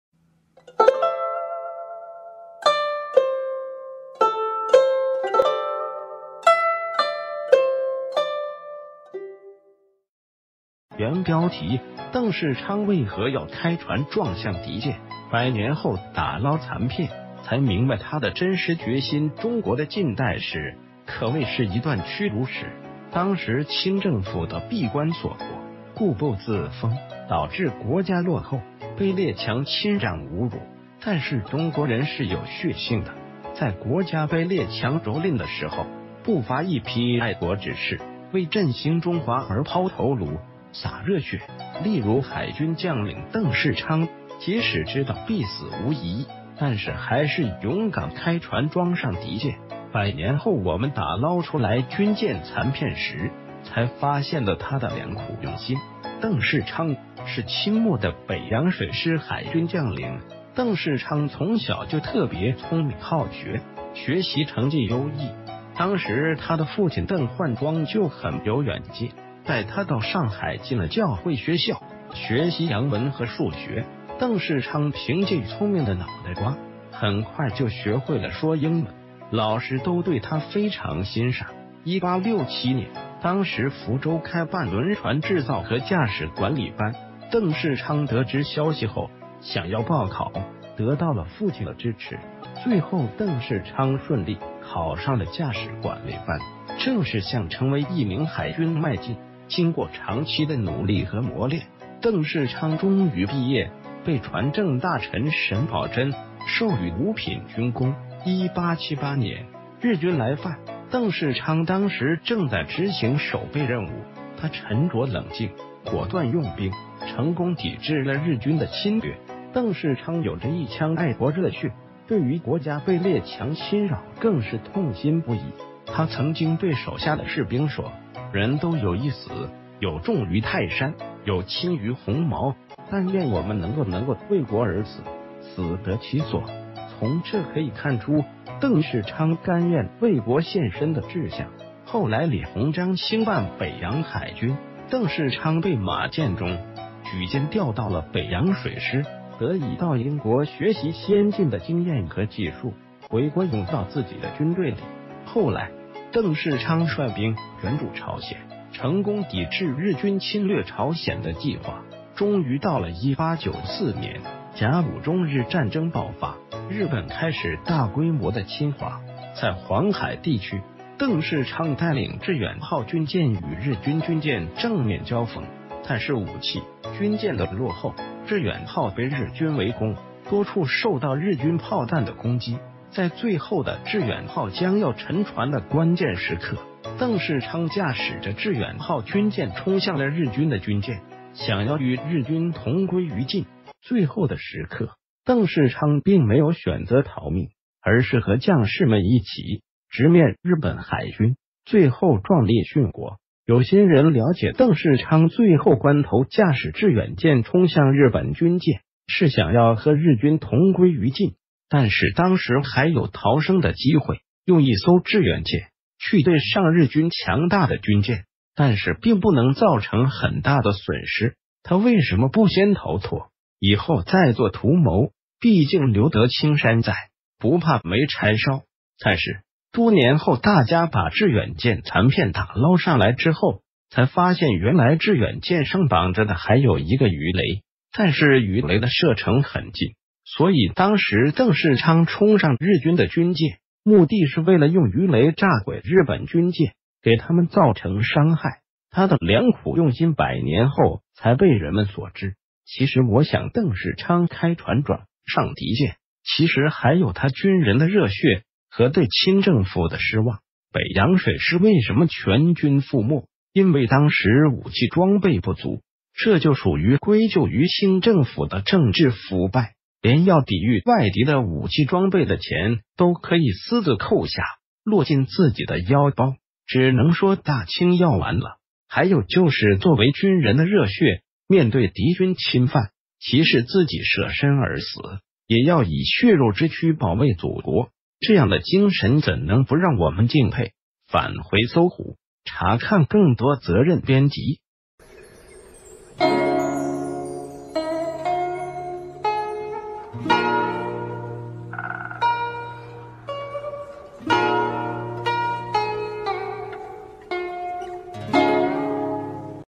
鄧世昌為何要開船撞向敵艦？百年后打撈殘片，才明白他的真實決心_搜狐歷史_搜狐網
原標題：鄧世昌為何要...